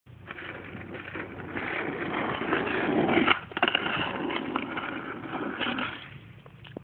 Ollie over skateboard 2
Me Jumping over a skateboard :D